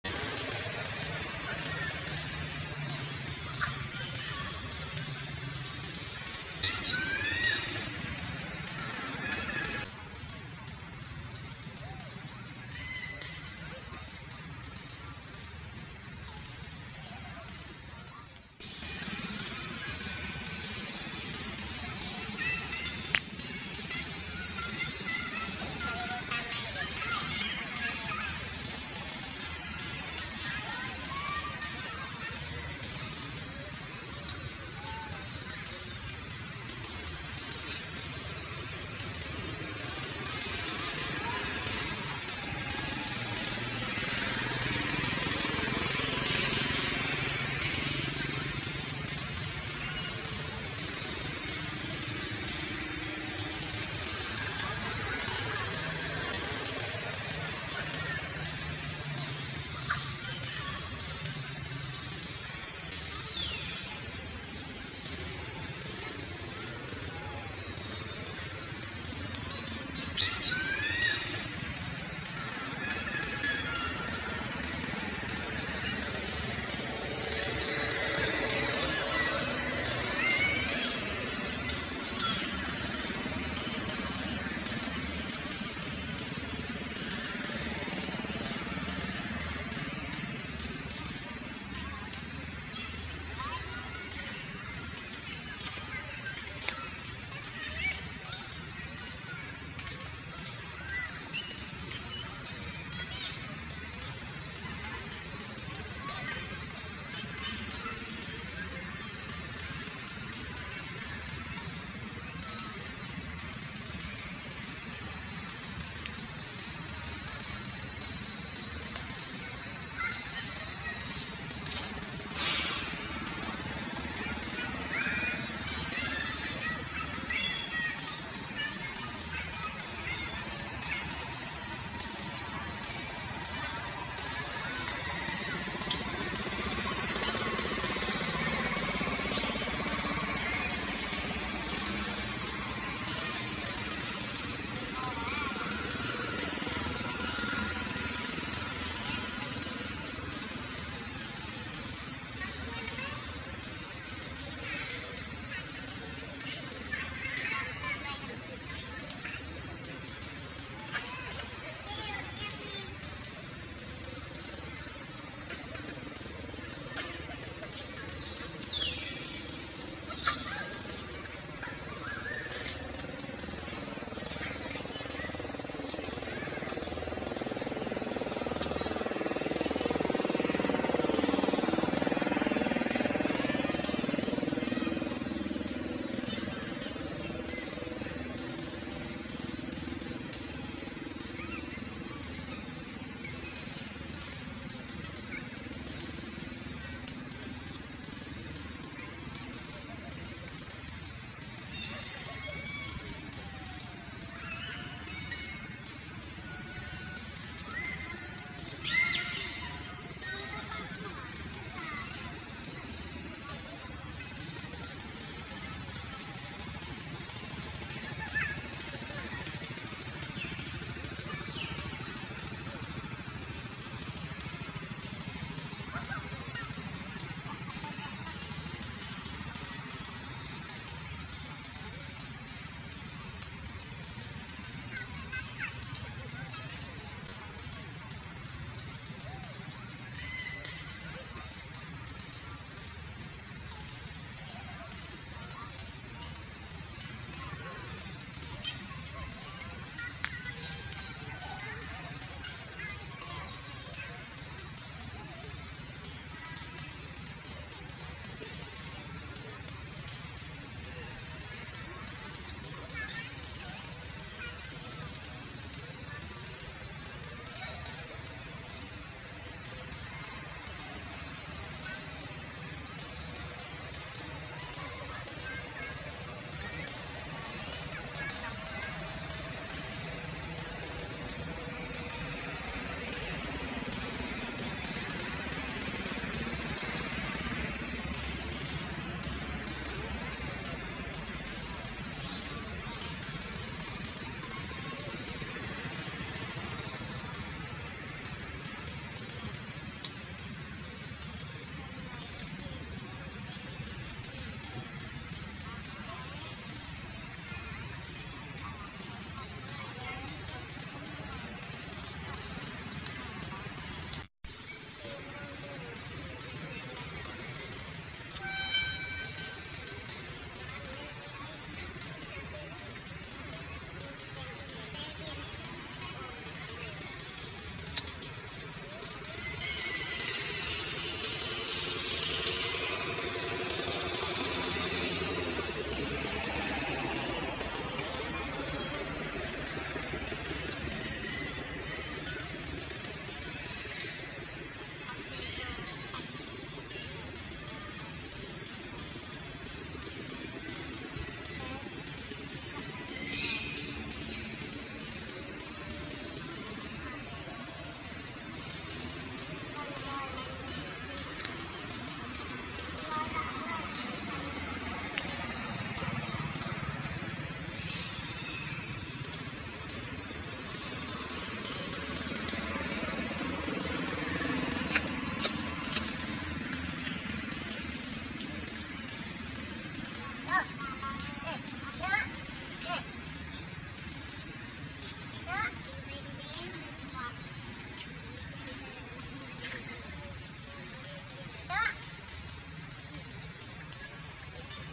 Mother monkey Rose allowed her baby monkey Robin playing with a little monkey and she has time also
Dear My Viewer This Video Telling About : Mother monkey Rose allowed her baby monkey Robin playing with a little monkey and she has time also